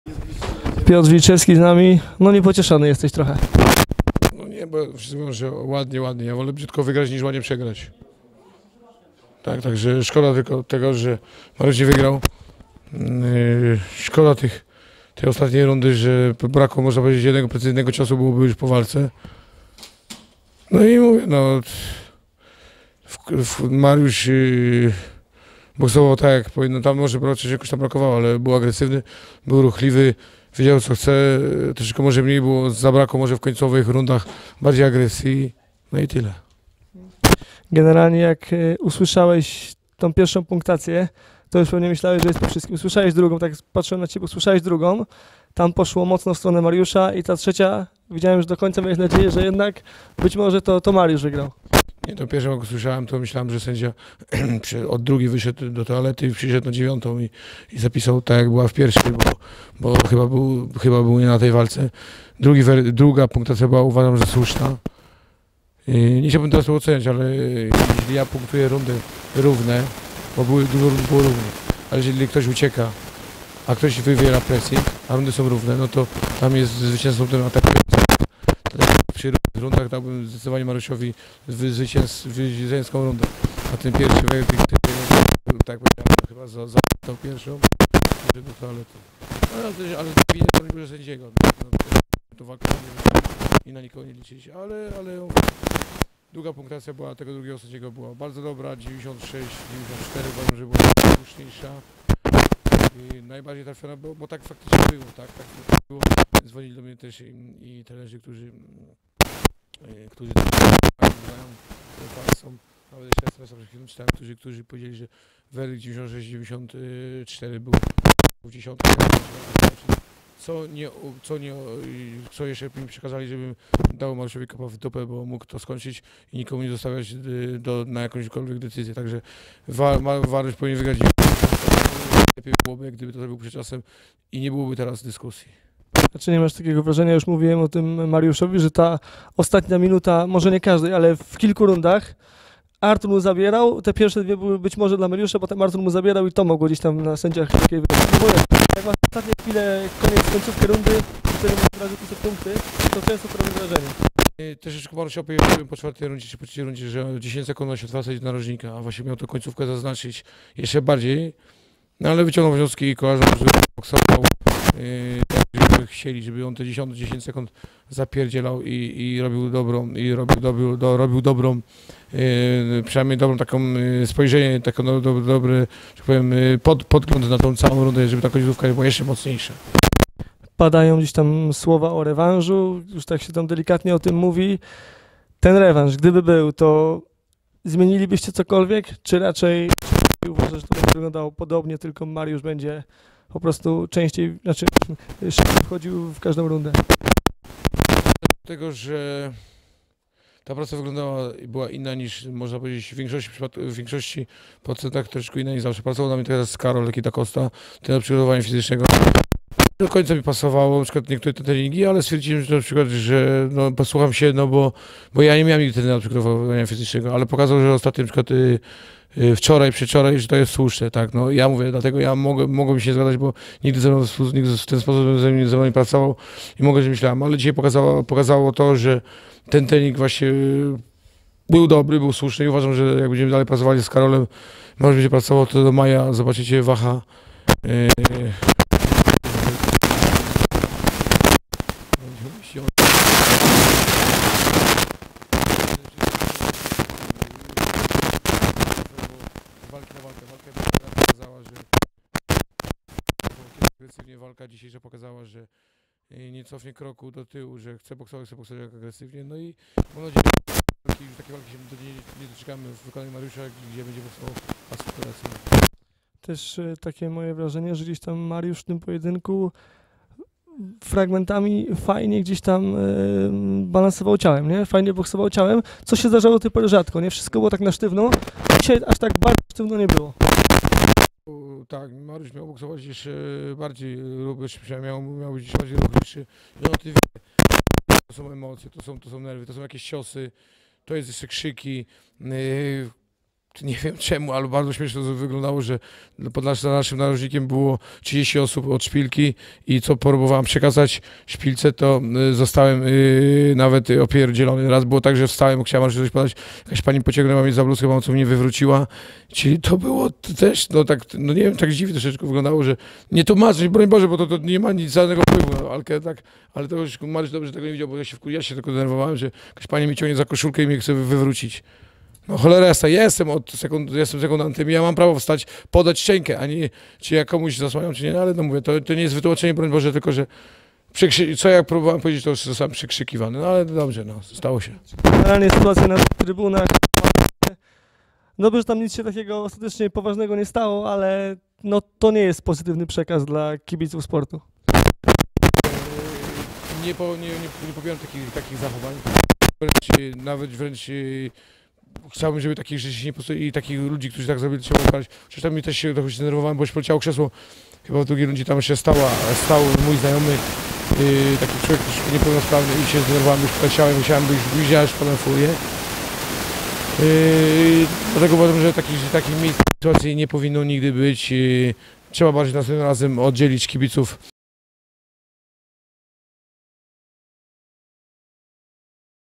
Wilczewski: Myślałem, że jeden z sędziów wyszedł do toalety
Prezentujemy rozmowę z Piotrem Wilczewskim po pojedynku Mariusza Wacha z Arturem Szpilką na Knockout Boxing Night w Gliwicach.